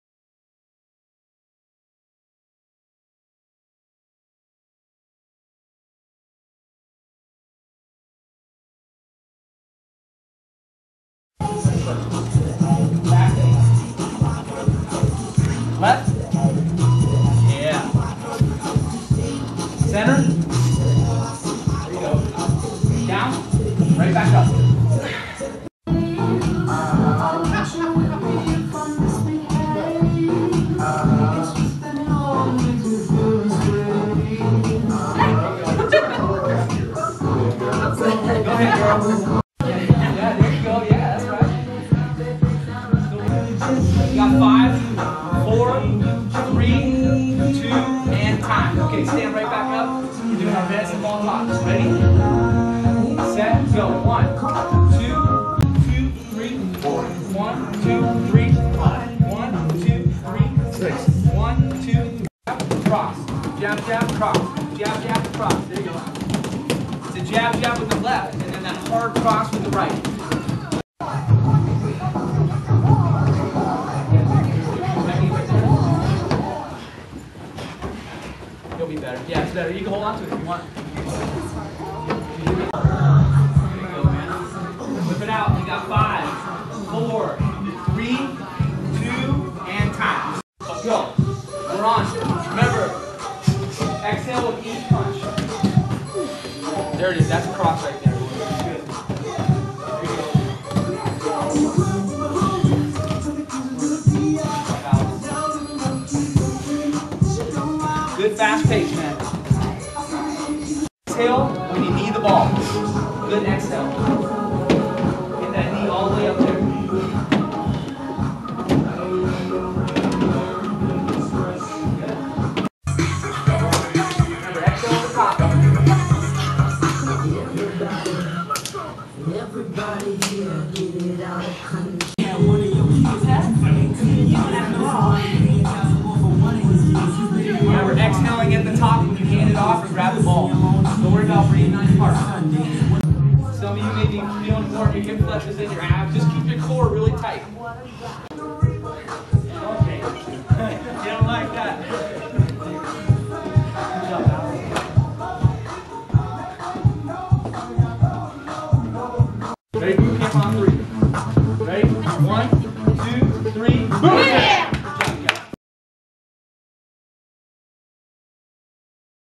Generation Fitness® Boot Camp
A compilation of 2 Generation Fitness® Boot Camp classes led by founder Josh Stanton.